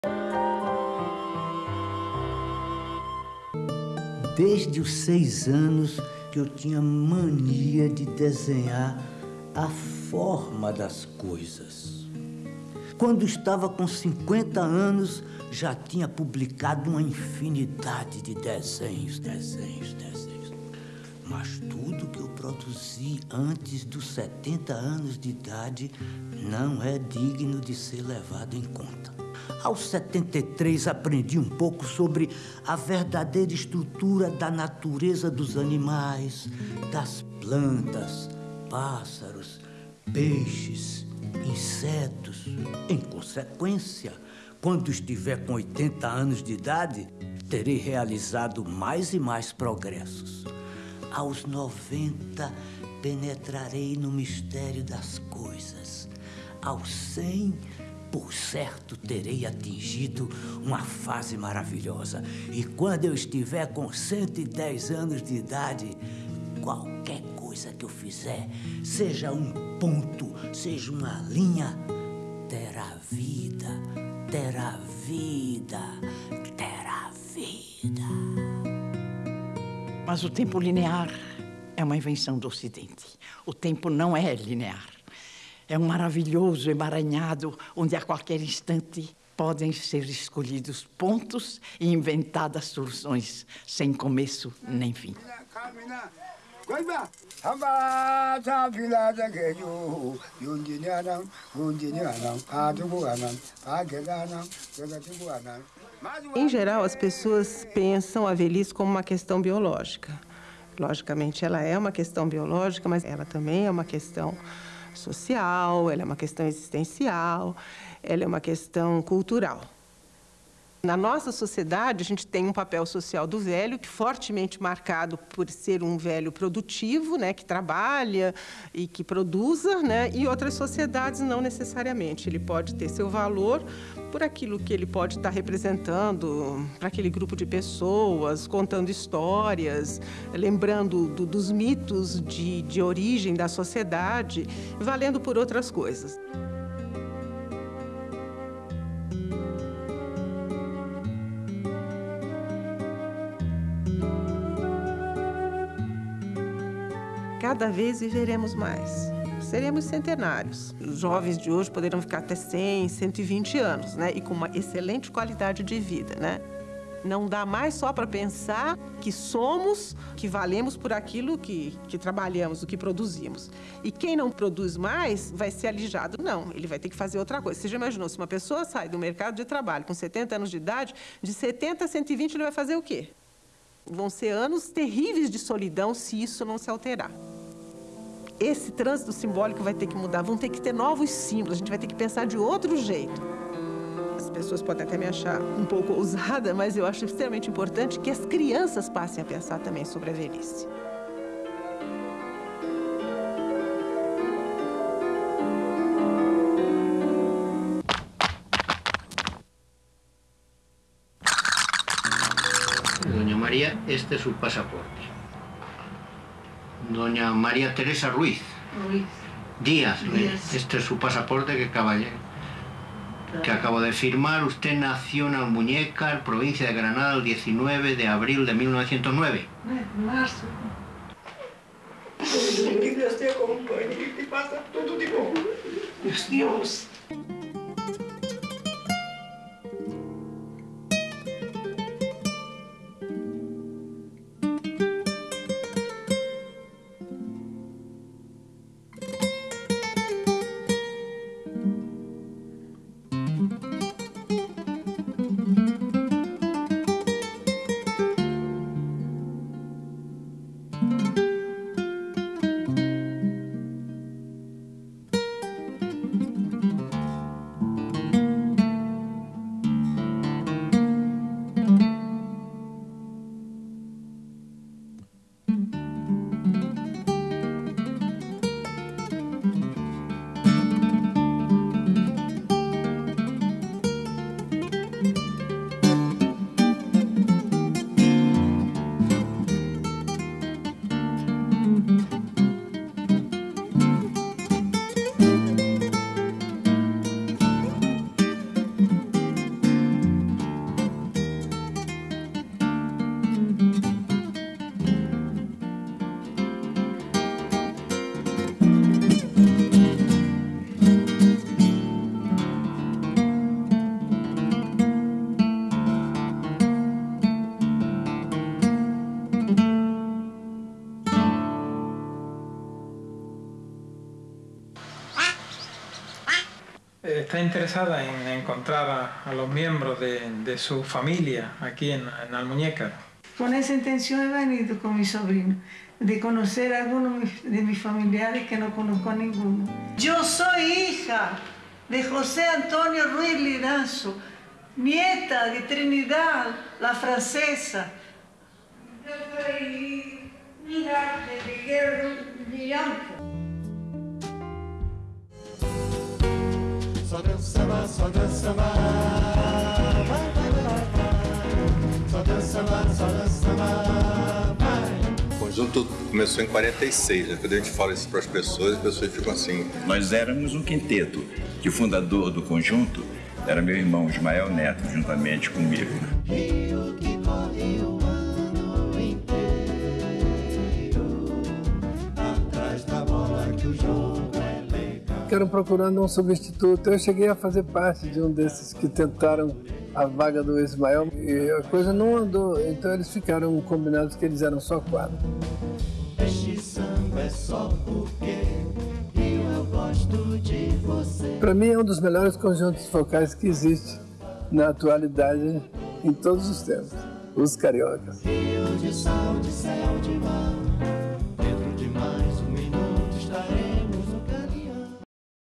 Programação Especial: Dia Internacional do Idoso
Programação em comemoração ao Dia Internacional do Idoso que reúne documentários, curta-metragens e especiais musicais que abordam temas relativos ao universo do idoso.